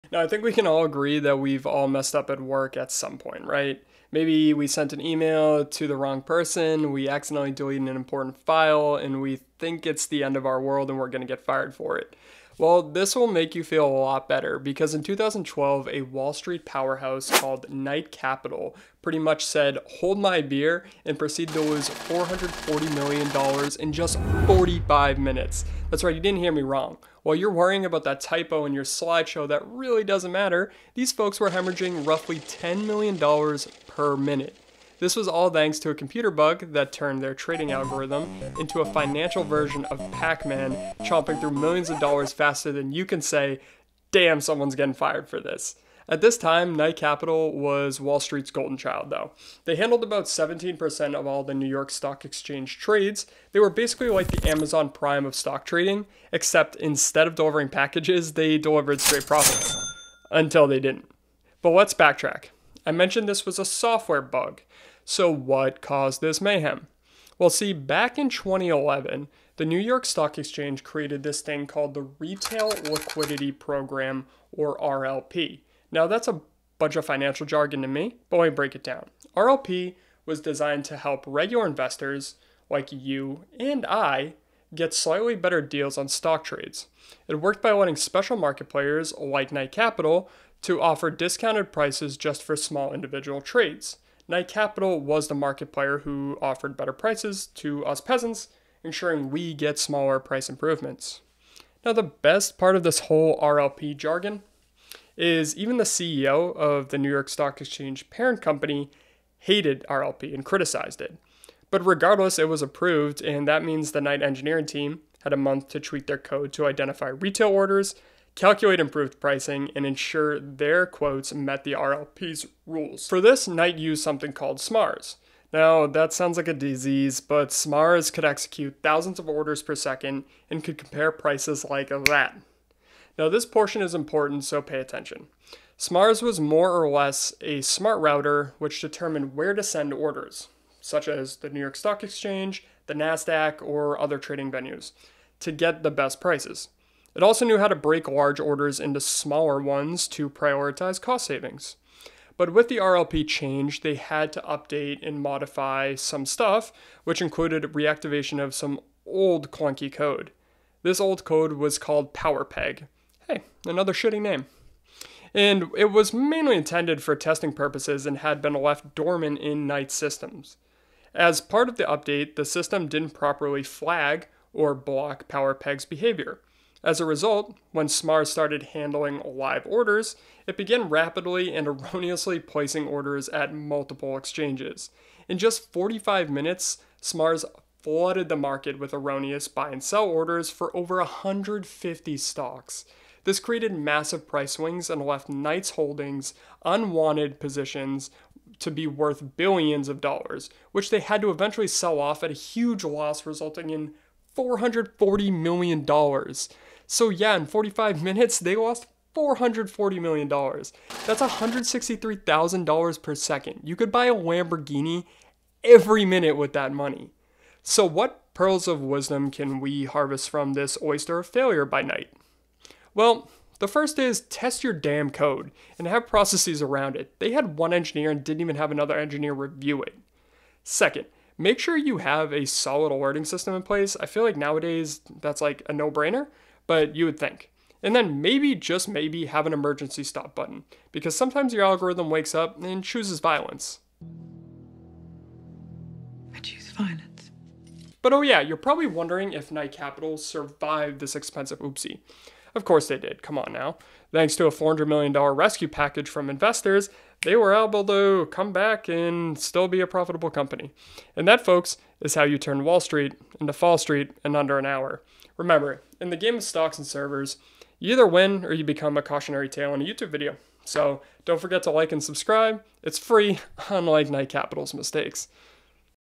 The $440M Bug That BROKE Wall Street
In 2012, Knight Capital Group went from Wall Street titan to financial disaster in just 45 minutes. One forgotten server, one old piece of code, and absolutely NO kill switch - this is the story of how a software bug caused one of the biggest financial disasters in trading history. $440 Million to be precise